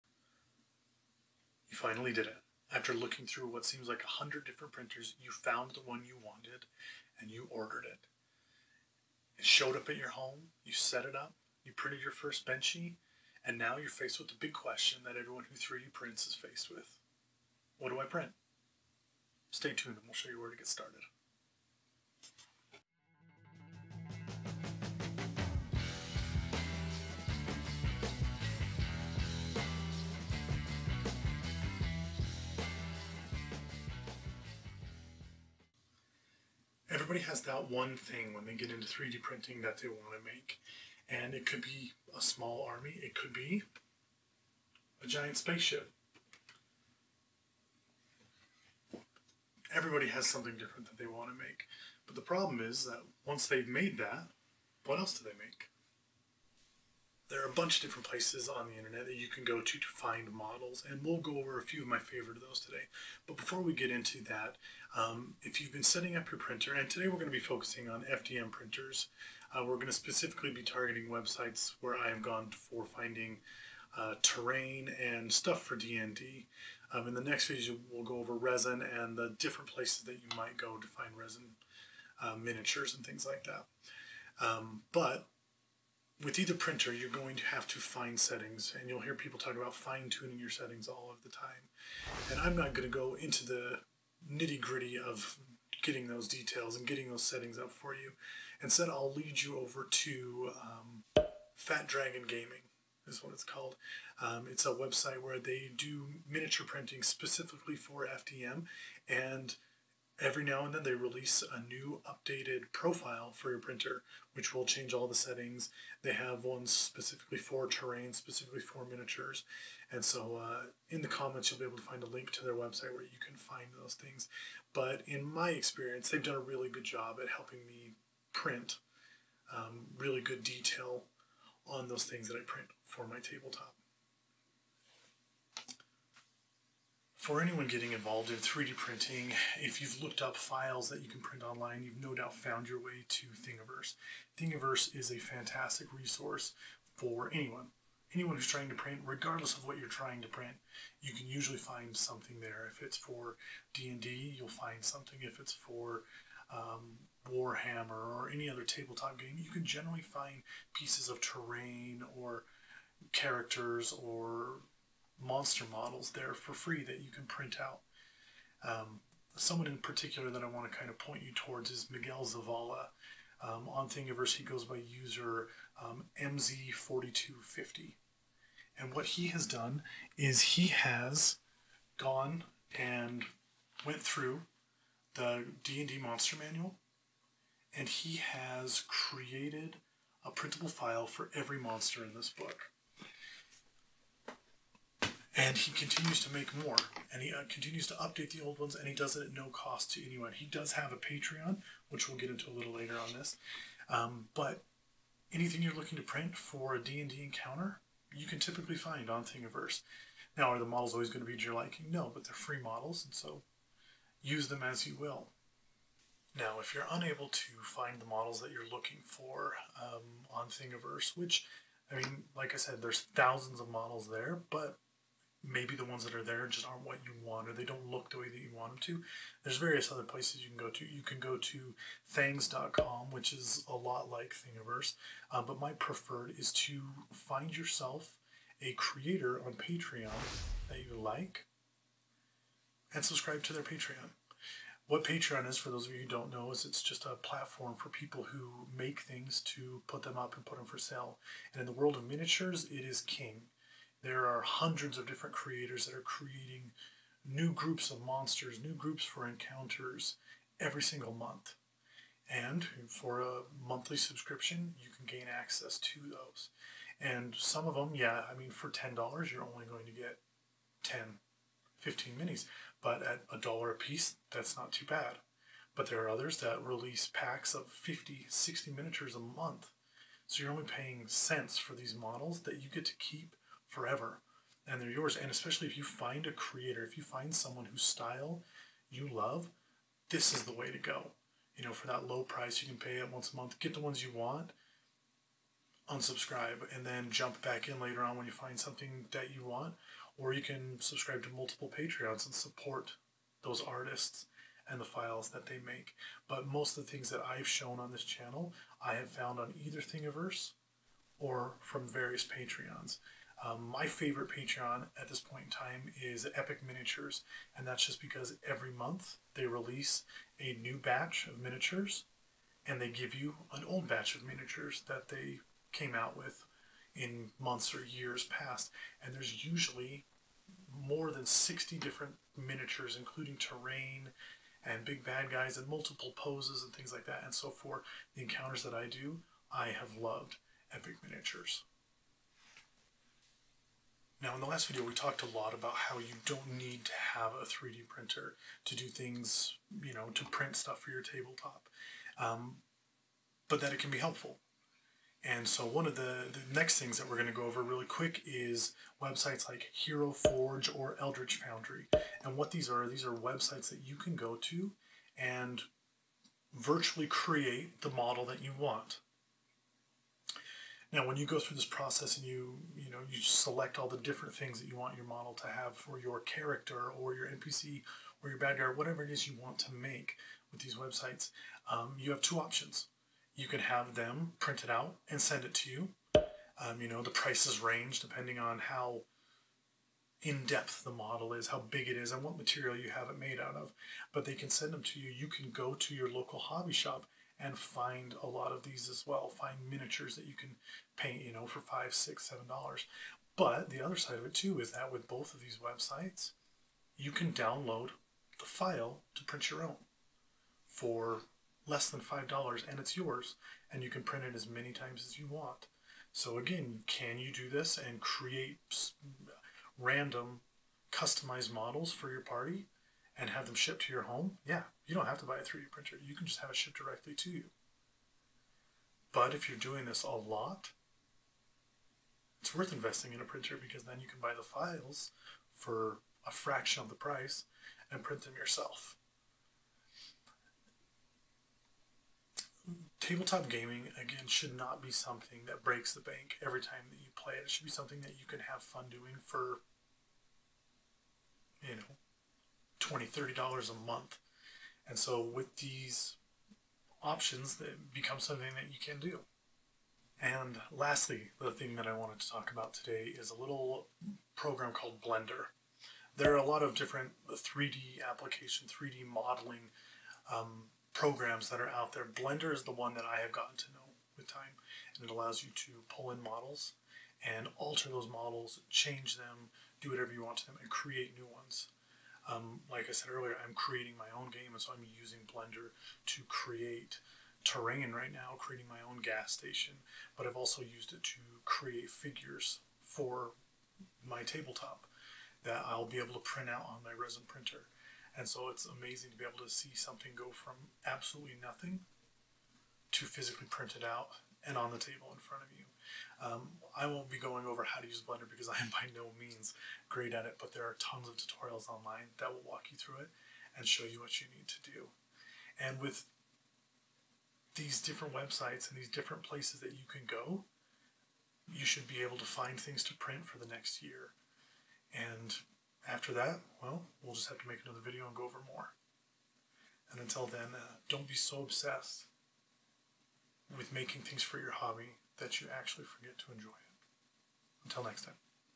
episode 2 What should I 3D print?
Now that you have a 3D printer what should you print? Where should you go to find things to print? We'll go over my favorite sites for tabletop gaming with an FDM printer.